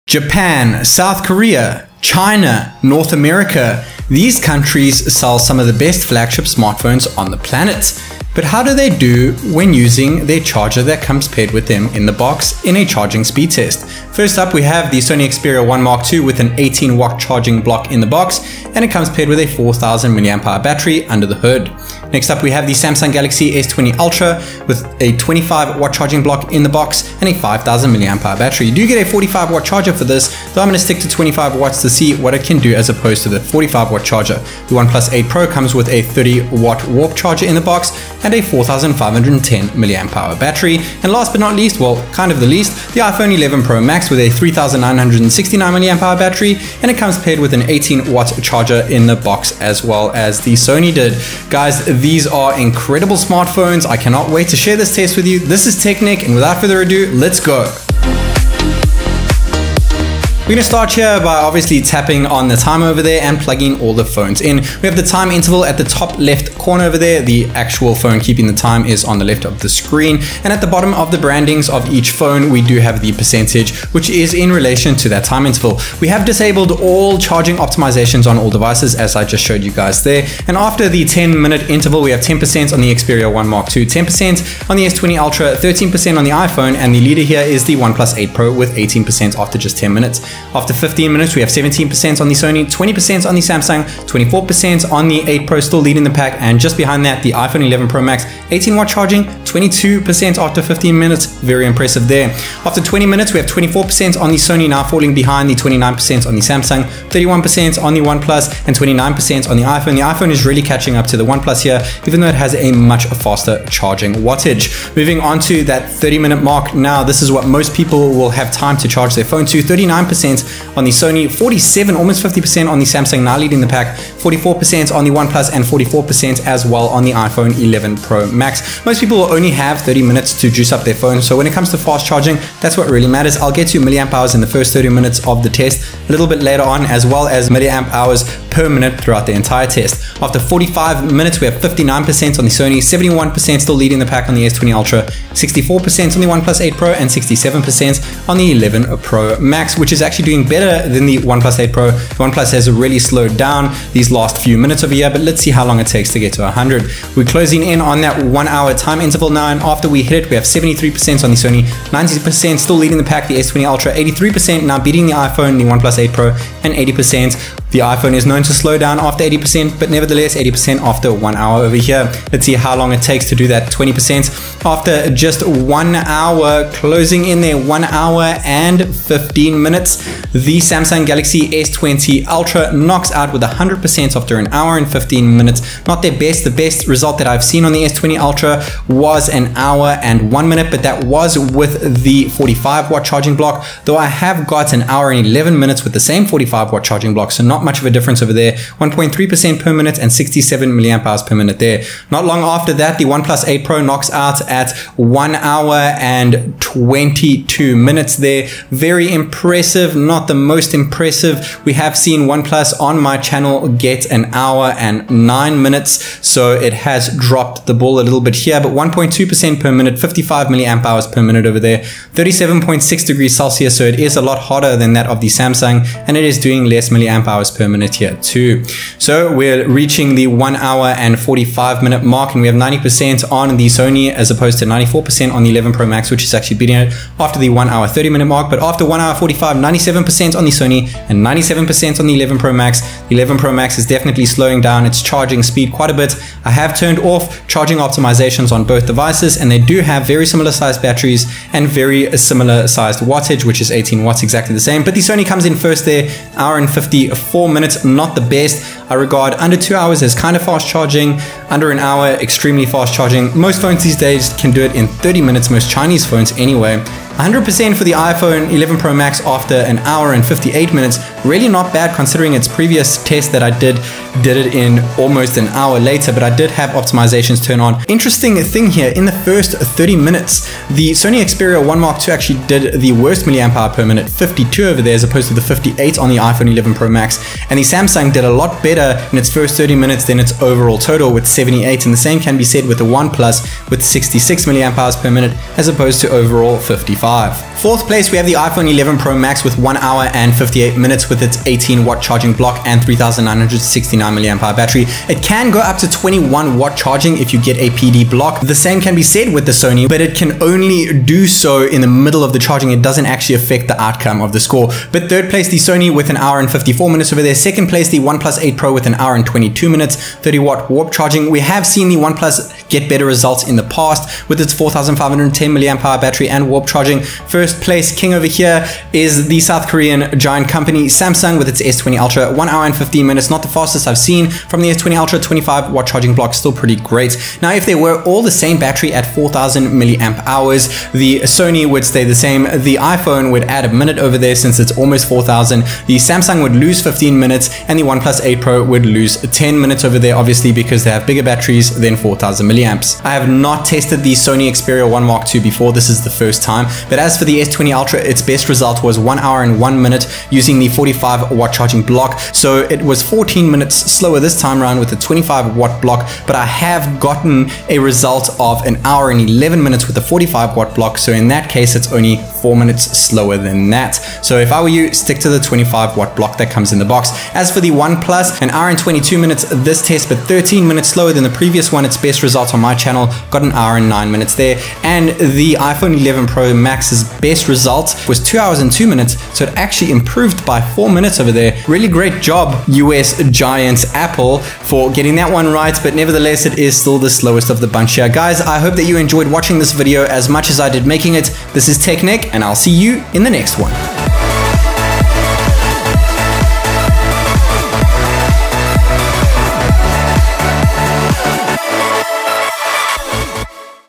BATTERY CHARGE TEST - Sony Xperia 1 ii vs S20 Ultra / OnePlus 8 Pro / iPhone 11 Pro Max
Japan, South Korea, China and USA make some of the BEST Flagship Smartphones around, but how do they Compete in a Charging Speed Test using the Chargers that these companies provided in the Box? 18W vs 25W vs 30W Charging Speed Test Between FOUR PREMIUM DEVICES! - Which Phone do YOU Think WILL Charge the FASTEST?

**[All Charging Optimizations have been Disabled]**

I compare Charging Speeds from 0-100% on all 4 devices! This includes battery charge mAh/min rate (first 30 min and full charge), battery temperatures, comparative stats to previous tests and some facts about fast charging ;)

00:00 - Introduction & Specs
01:00 - Charging Speed Test (0-100%)
04:31 - mAh/minute of 30 Min Charge & Full Charge
04:57 - Results
05:37 - Results if all Phones had the same 4000mAh Battery
05:53 - mAh/min Results, Results compared to BEST previous tests & Final thoughts